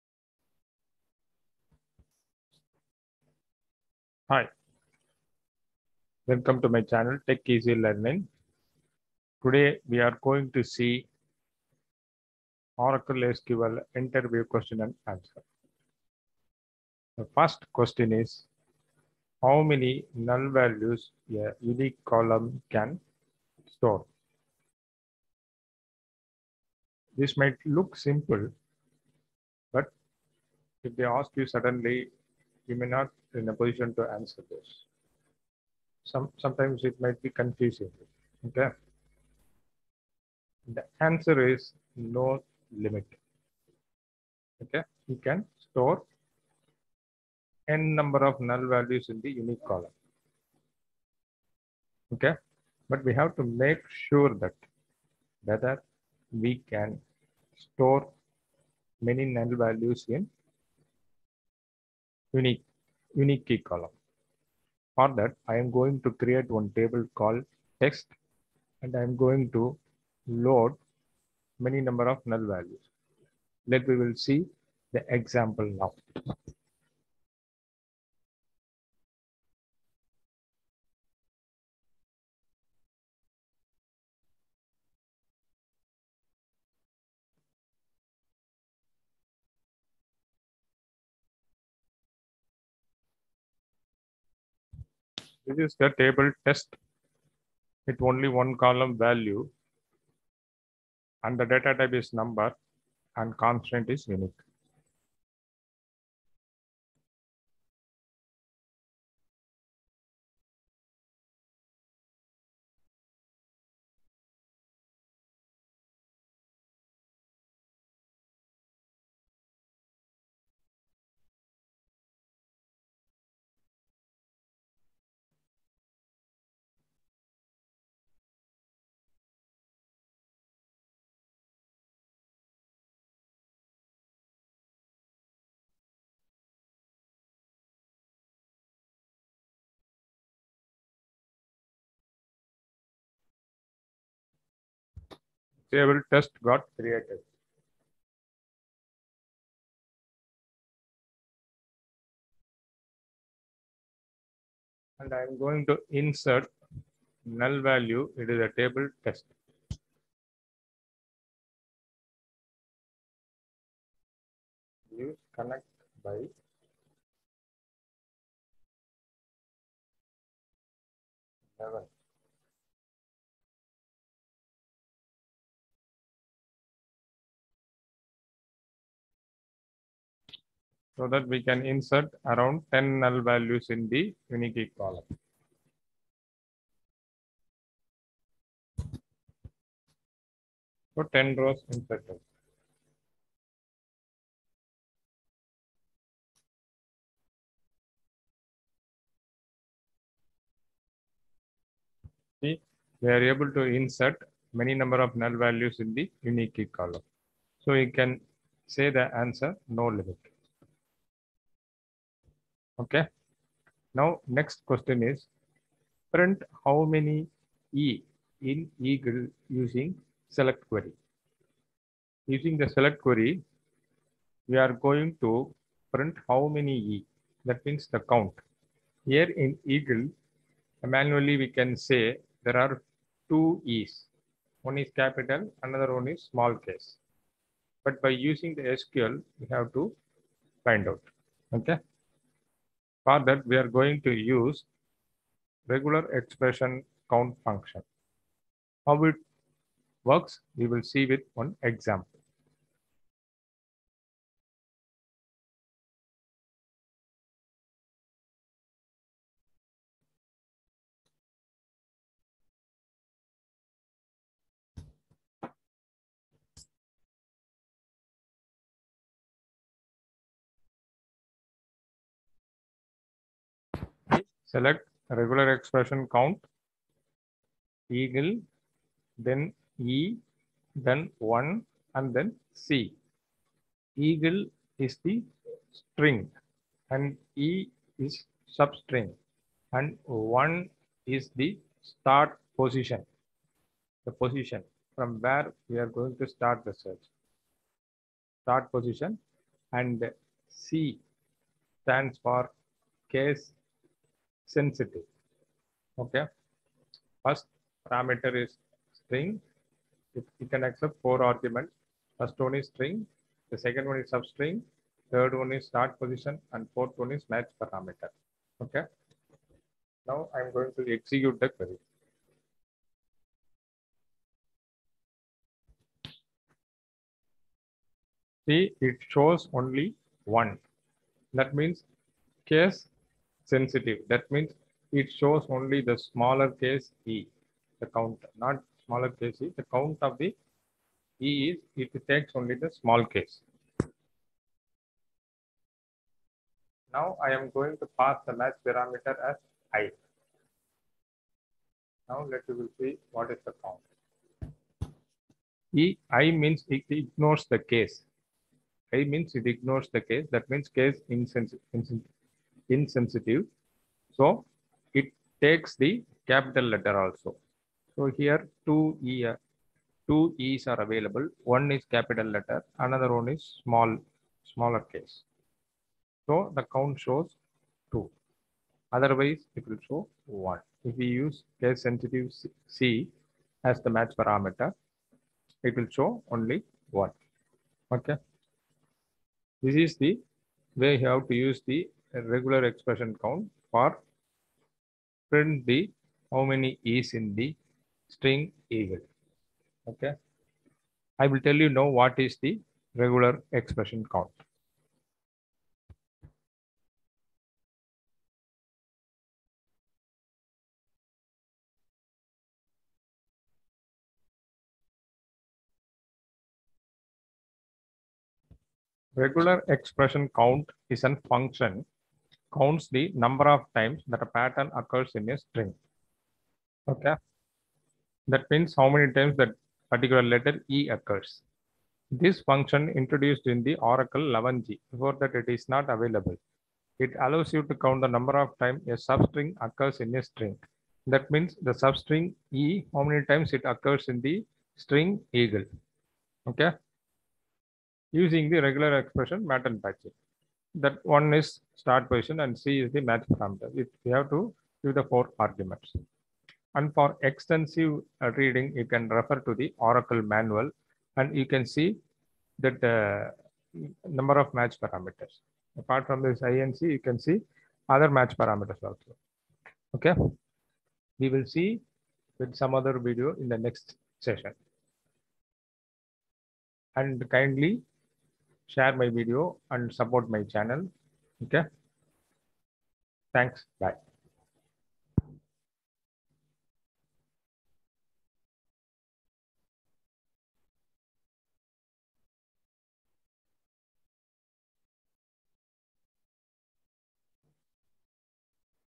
Oracle Real Time Scenarios, Oracle SQL, SQL, REGEXP_COUNT()
Oracle Real Time Scenarios, Oracle SQL, SQL

REGEXP_COUNT function counts the number of times that a pattern occurs in a string. This function, introduced in Oracle 11g, will allow you to count the number of times a substring occurs in a string using regular expression pattern matching.